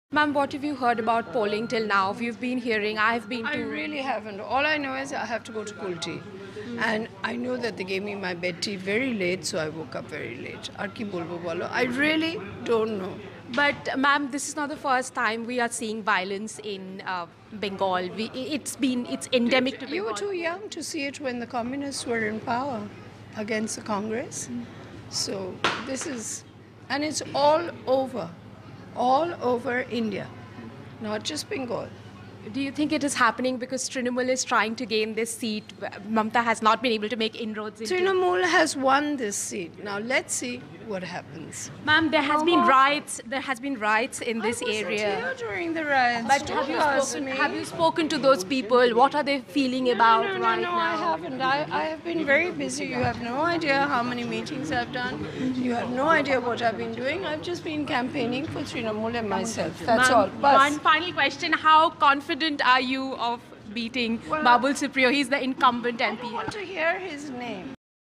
Moon Moon Sen when asked about violence in Asansol by TMC goons shamelessly defends it.
Pathetic. TMC candidate Moon Moon Sen looks unhinged. On asked of poll violence by TMC says she got bed tea late so came late for polls. Asked of Asansol violence again, she shamelessly defends it says Left-CONgress poll violence was far greater! Vote for Babul Supriyo.